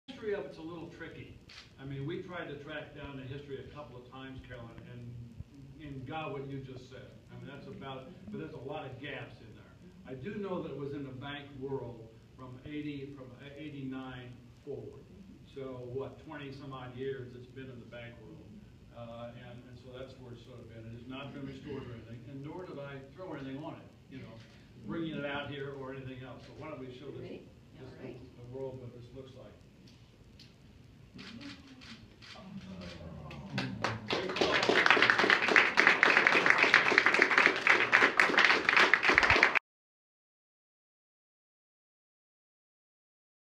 Thomas Hart Benton's "The New Fence" Returns to Westminster College [Highlight]
Jim Hance '66 presents the gift of Thomas Hart Benton's "The New Fence," originally given to Sir Winston Churchill at Westminster in 1946, to the College. Dec. 4, 2014